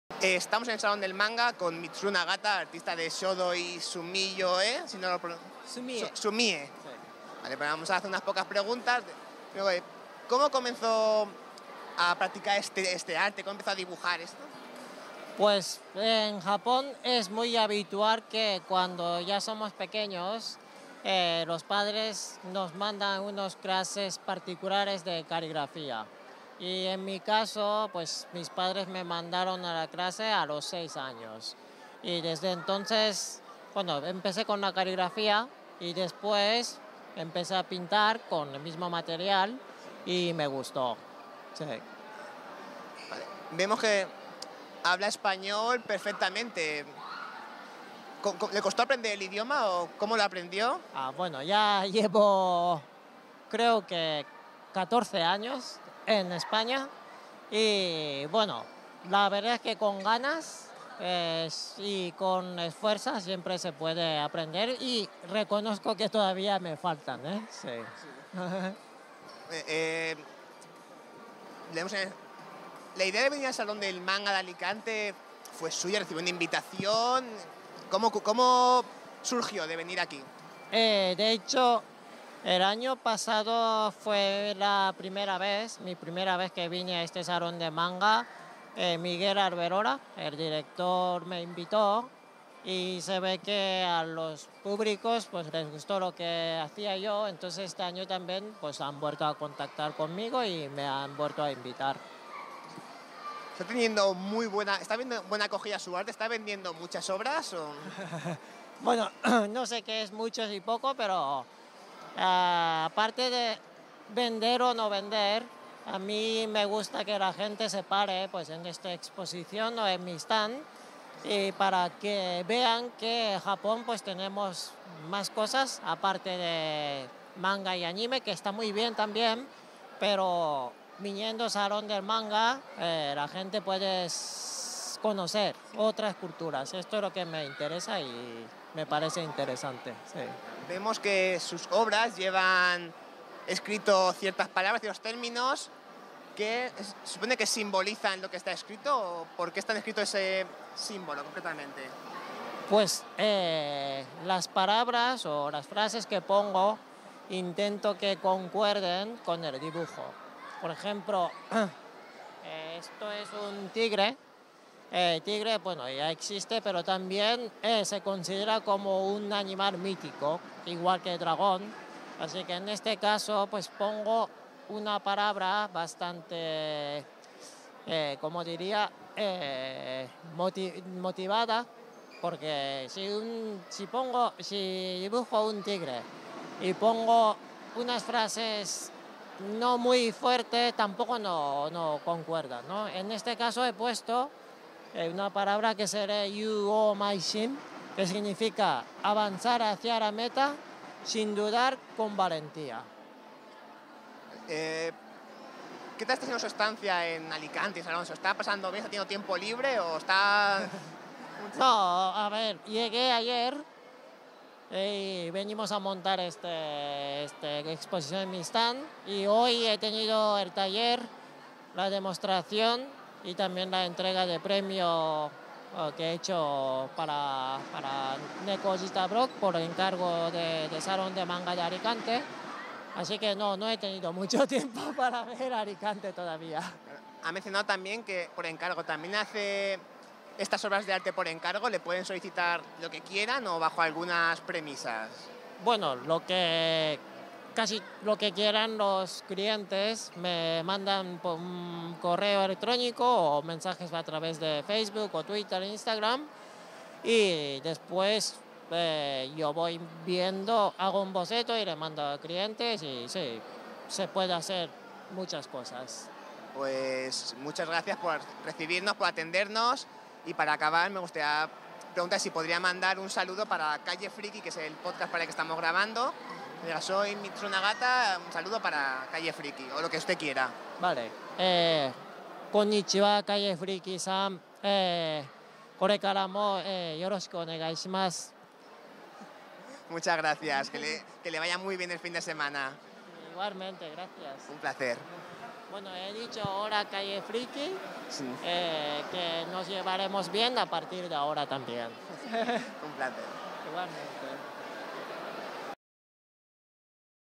Calle Friki Entrevista a Mitsuru Nagata artista Sumi-e y Shodou
Interesante entrevista a al artista japones afincado en Barcelona.